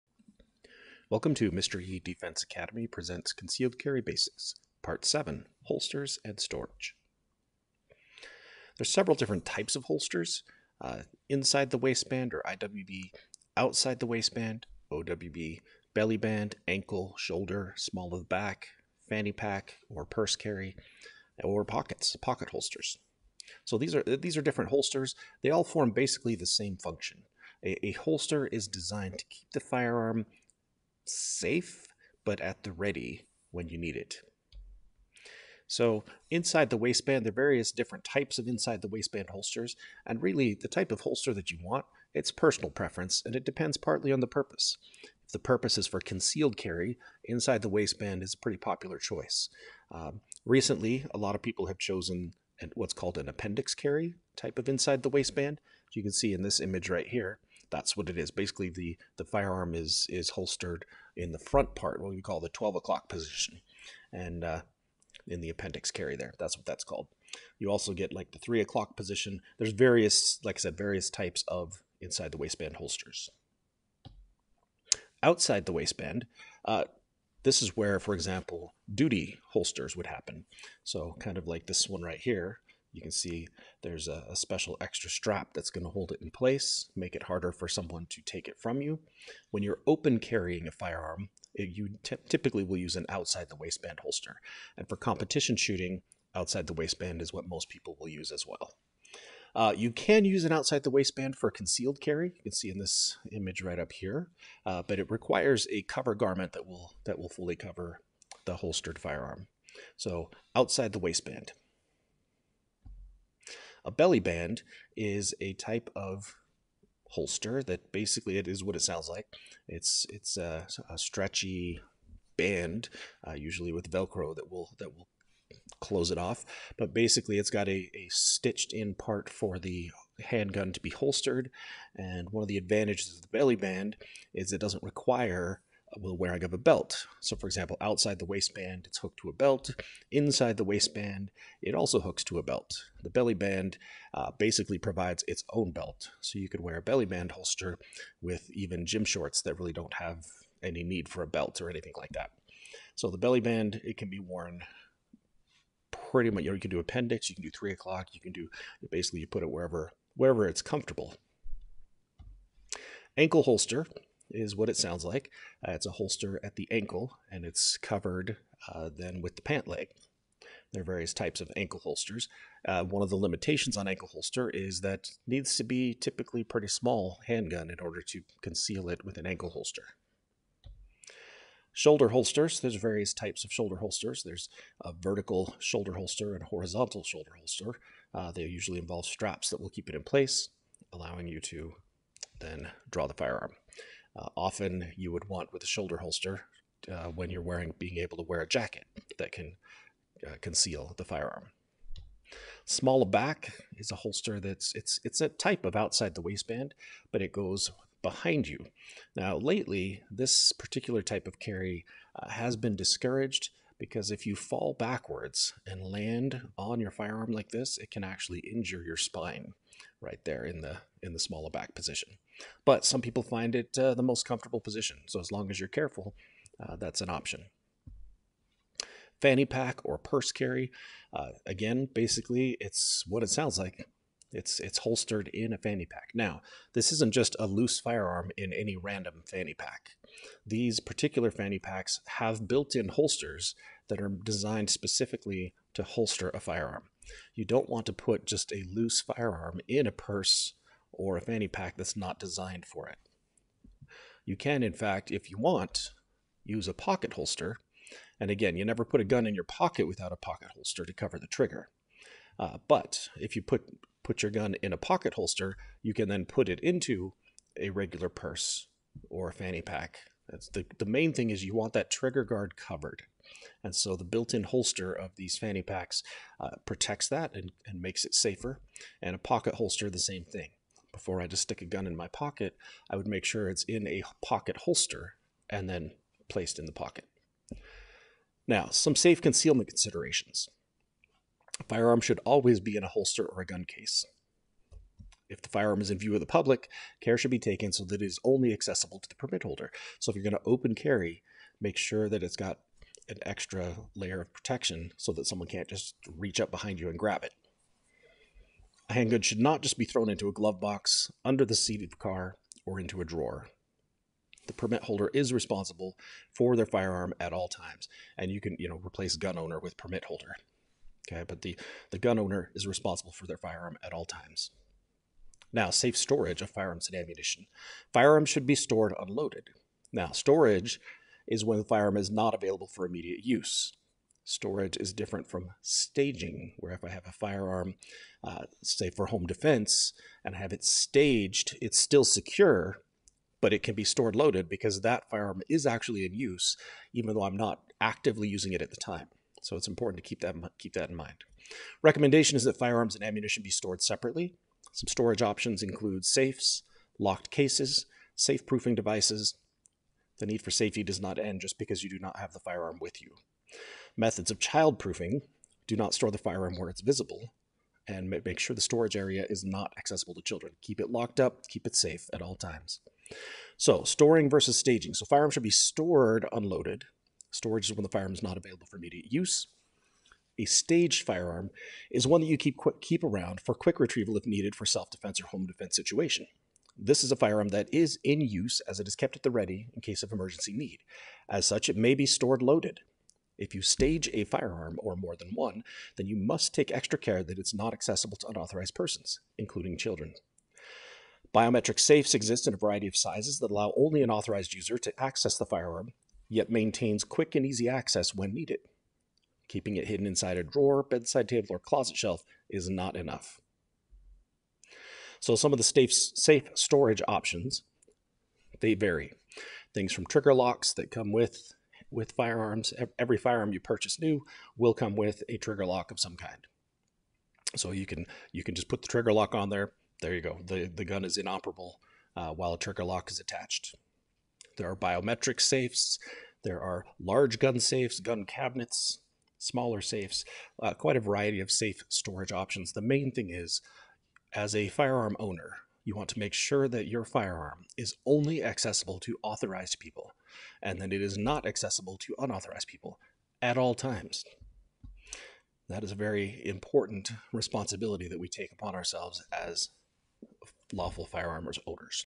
Concealed Carry Basics 7 Holsters and Storage
This video in our concealed carry basics series addresses the variety of holster options available.  The importance of safe storage is also discussed.